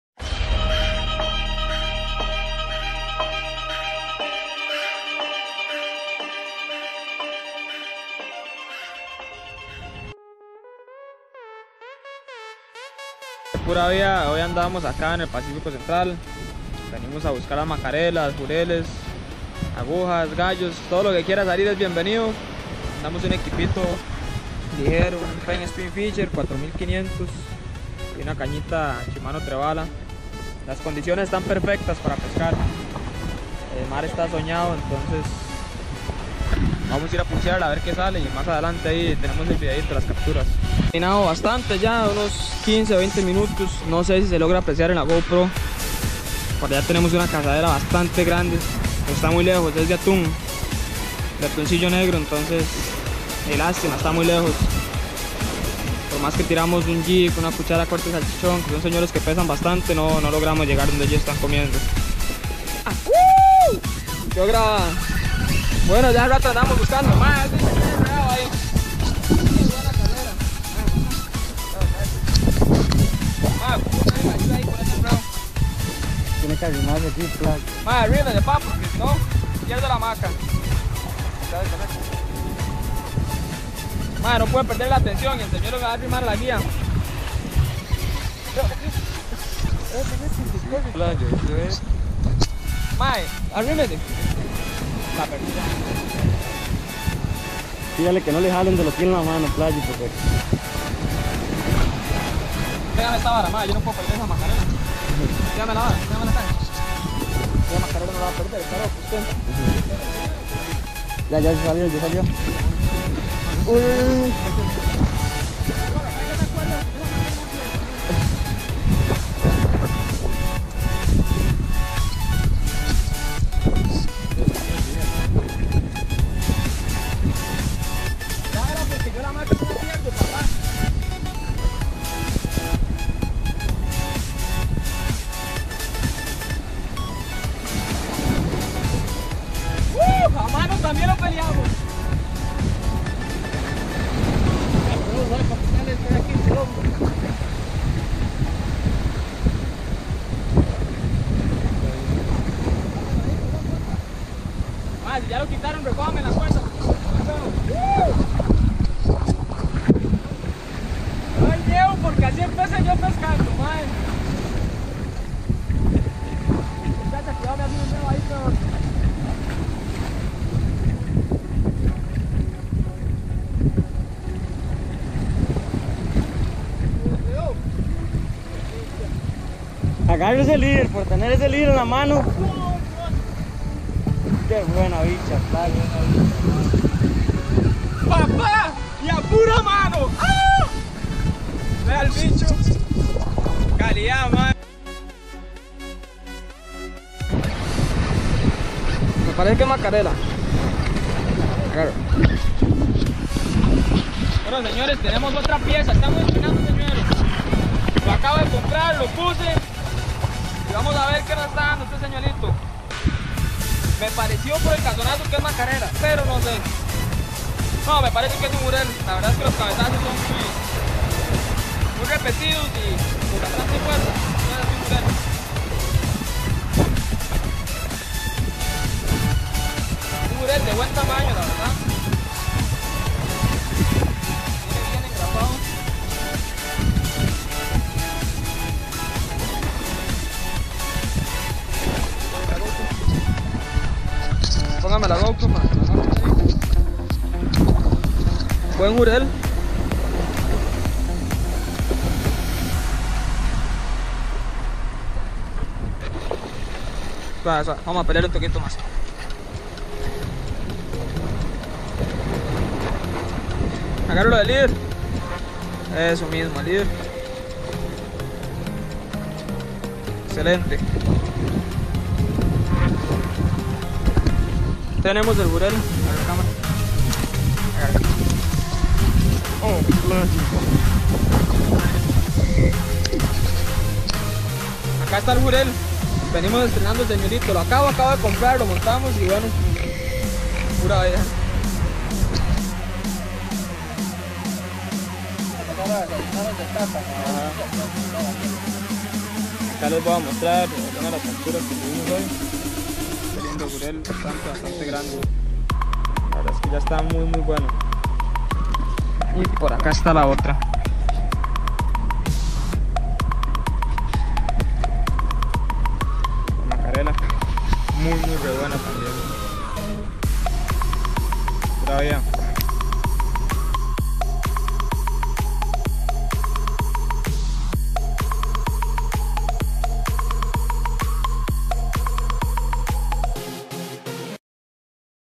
Shore fishing In Costa Rica , Mackerel and Trevally
Un buen dia de pesca , una macarla de 12 lbs aproxi. Y un Excelente Jurel 🔥🎣🇨🇷